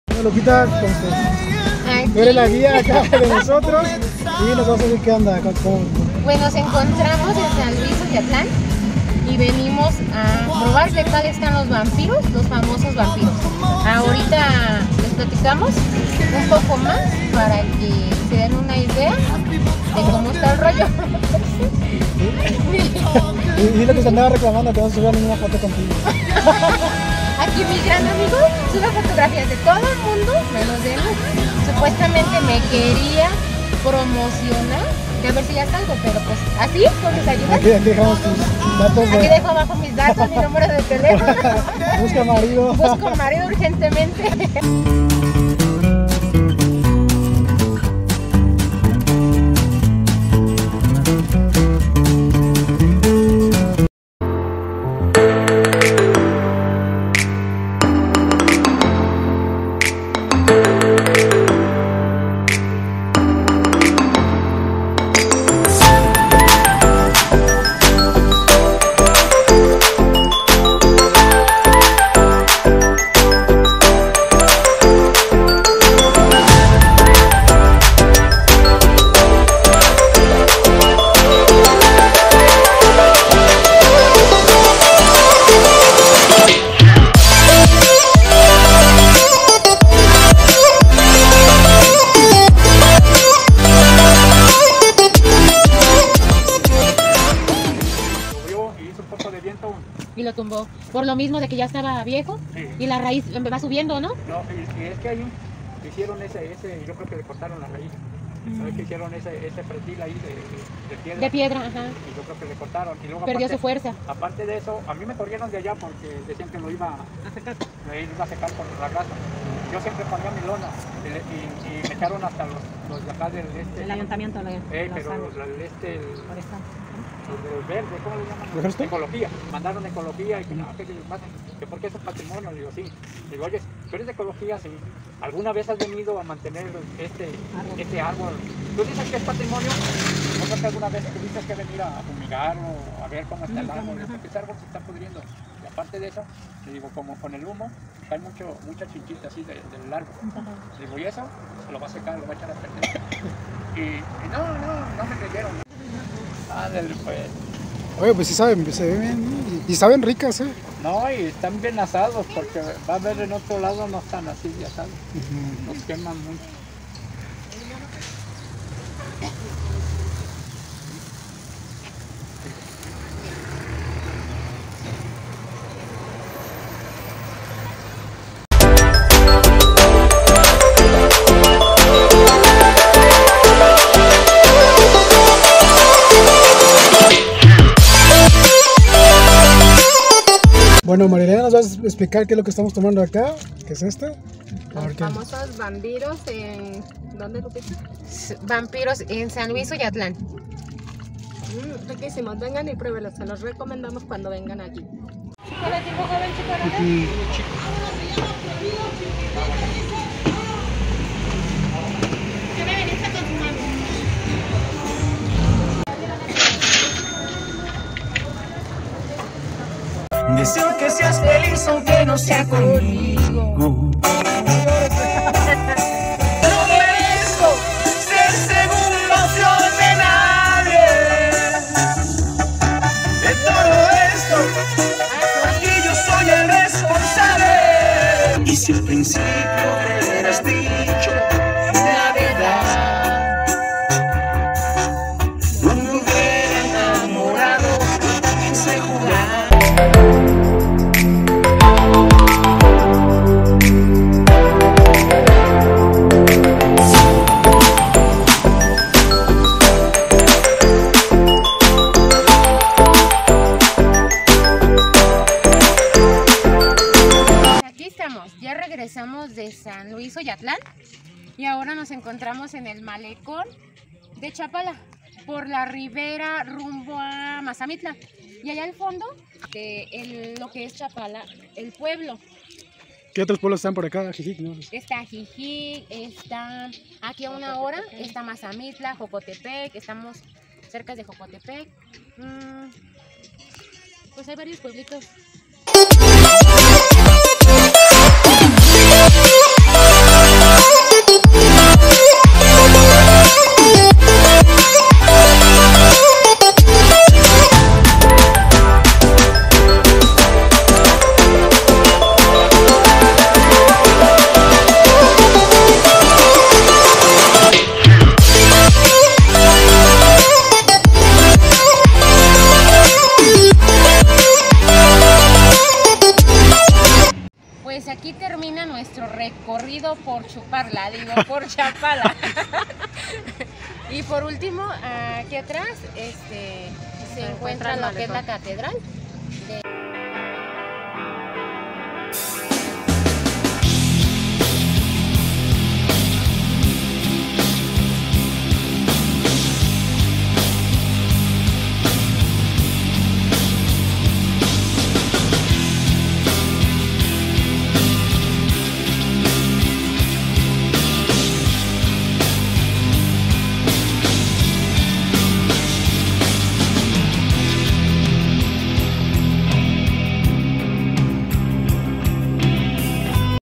Chapala Jalisco y la parte de la rivera del lago de Chapala(vampiros de soyatlan Jal)
viajamos de Guadalajara a Chapala, recorriendo toda la rivera hasta llegar a soyatlan Jal y probar los famosos vampiros, de paso pollos al carbón, con la guía de mi amiga Lupita, oriunda del Grullo Jalisco.